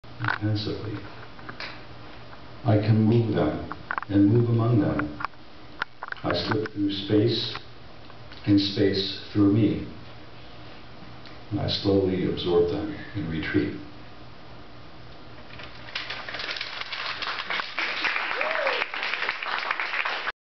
Poetry Slam 5
Josh Ostoff reads a poem.